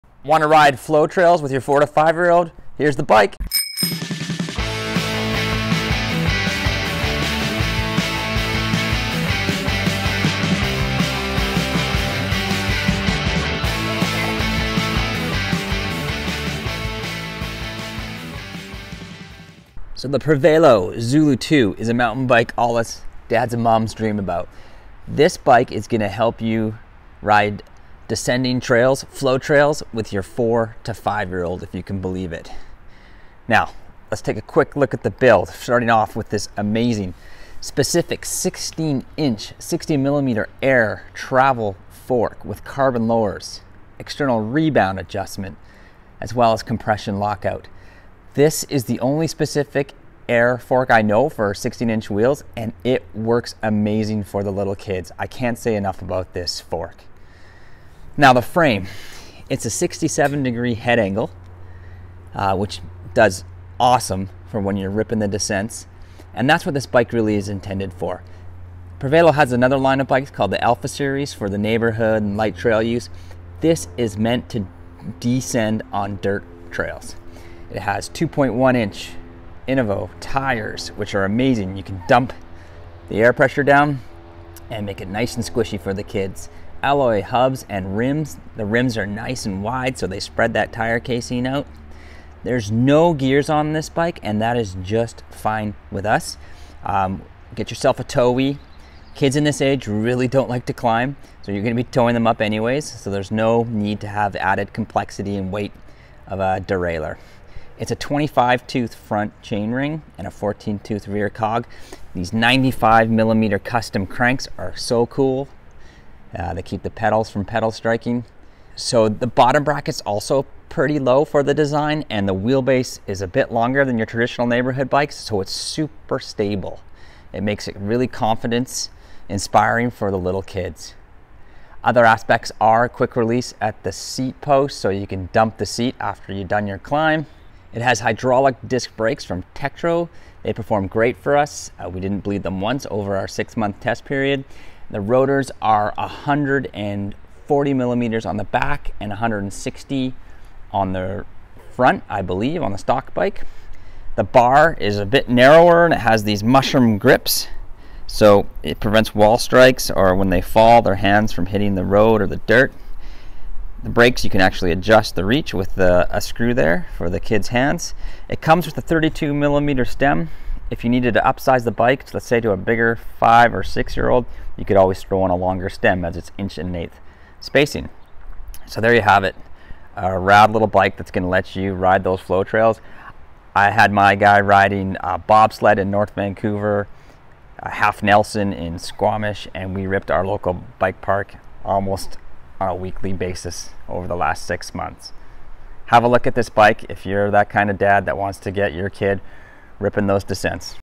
Prevelo Zulu 2 Heir Review
Weight: 18 lbs | MSRP: $799 | Min-Max seat height: 18.1"-25" | Where to buy

Yes!!! We have been waiting for this bike for some time now. Exactly what we wanted at a great price, this 16 inch specific mtb is drool worthy for all us BD's and BM's. It must be said that the carbon lowers rebound adjustable  air fork is amazing. It is specific to 16 inch wheels and works, it actually works! The rest of the bike is tricked out with a 66 degree head angle, hydraulic disc brakes, 2.1 inch wide Innova tires and weighs in at 18 lbs. No cassette or derailleur option but that is fine with us as we prefer a tow whee over the added weight and price of an unused geared drive-train. My four year old has spent a lot of time on this bike and I am blown away by the terrain and skills he is mastering.